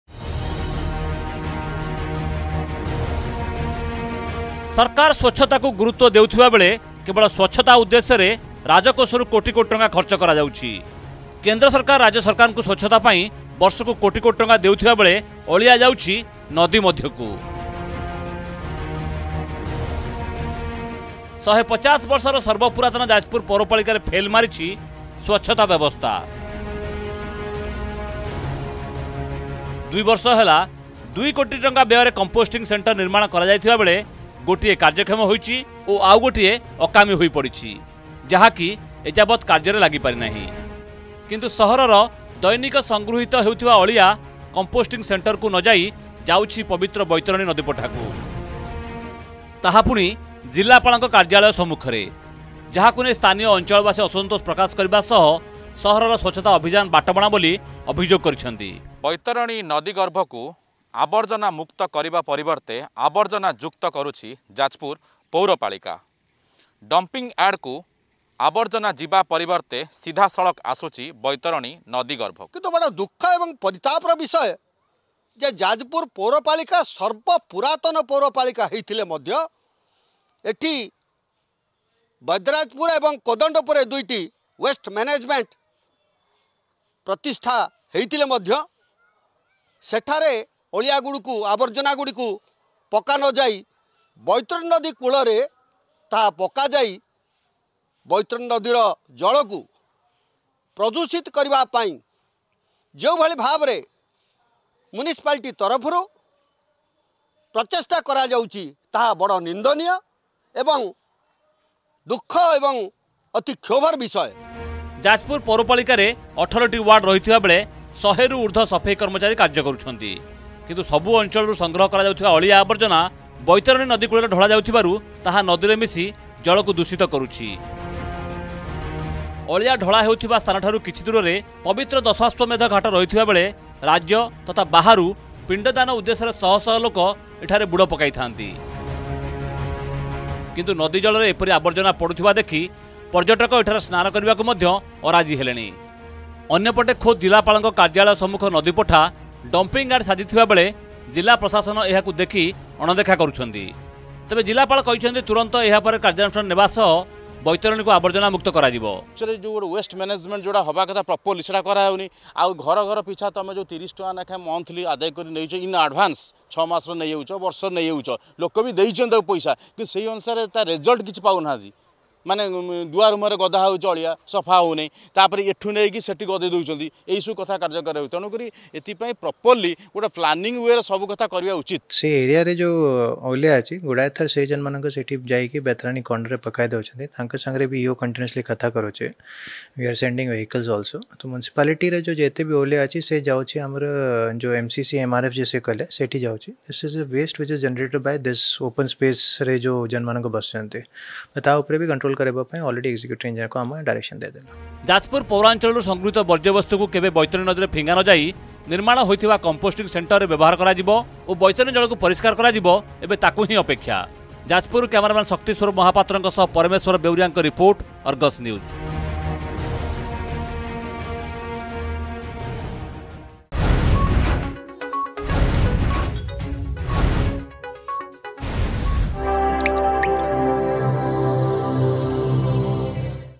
ଯାଜପୁର ପୌରପାଳିକା ପାଇଁ ଅପରିଷ୍କାର ହେଉଛି ପବିତ୍ର ବୈତରଣୀ ନଦୀ
ଯାଜପୁର ପୌରପାଳିକା ପାଇଁ ଅପରିଷ୍କାର ହେଉଛି ପବିତ୍ର ବୈତରଣୀ ନଦୀ। ପୌରାଚଂଳରୁ ଉଠାଯାଉଥିବା ଅଳିଆ ଆବର୍ଜନାକୁ ବୈତରଣୀ ନଦୀ ଗର୍ଭରେ ପକାଯାଉଥିବା ବେଳେ ନିର୍ମାଣ ହୋଇ ମଧ୍ୟ କାର୍ଯ୍ୟରେ ଲାଗୁନି କମ୍ପୋଷ୍ଟିଂ କେନ୍ଦ୍ର।